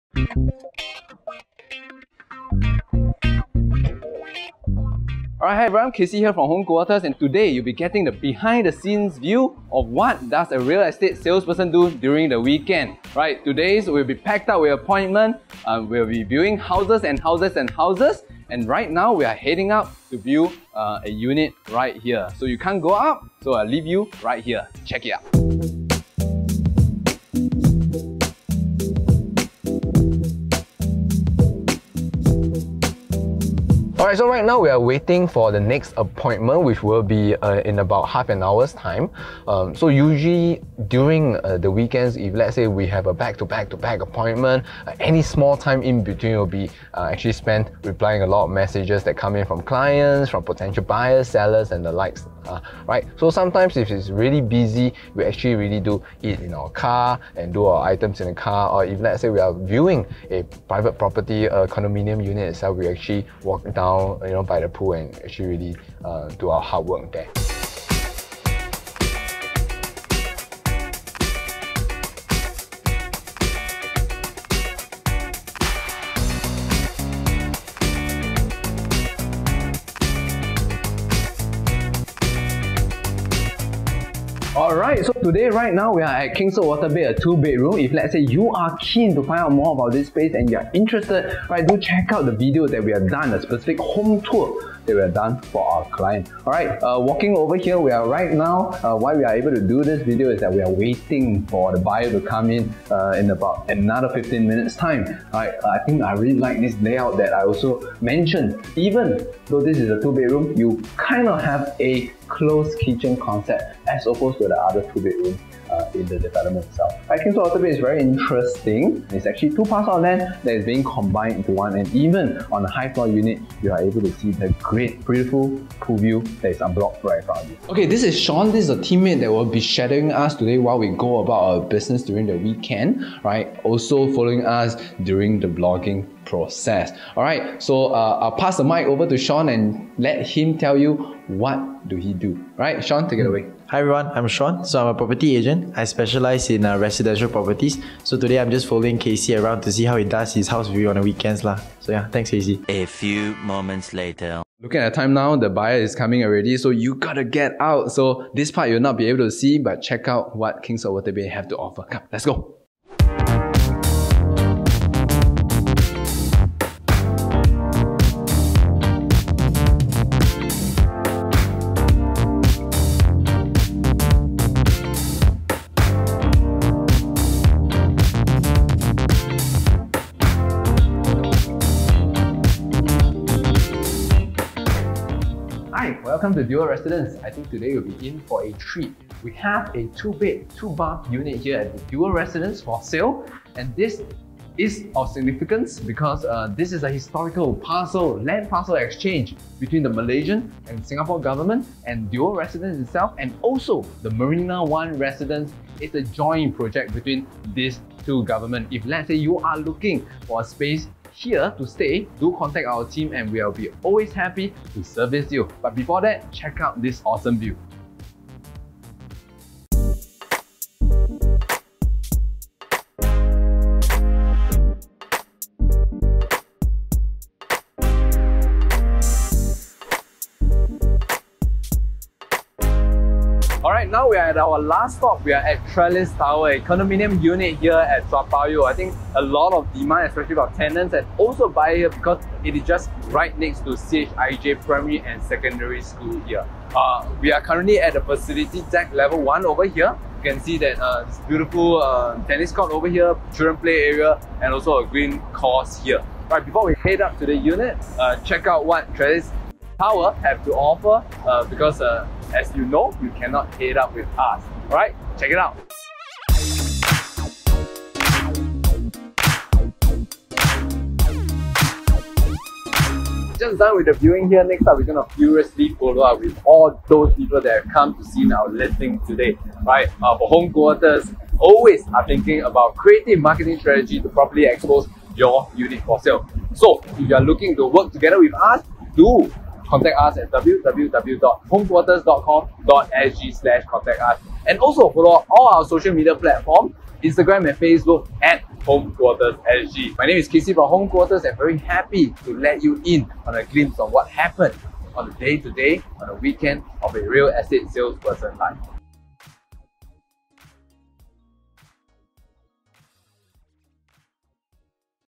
10 hours with a Singapore Realtor! | Home Quarters Vlog Ep 1
Hi everyone! Today we are going to show you what we at Home Quarters do during the weekends and let you have a sneak peek into the life of a real estate salesperson.

Ever wonder what a Singapore Real Estate Salesperson do? Click play to find out!

Some of the segments will be cut out as we will not be filming prospective buyers while they are doing the viewings.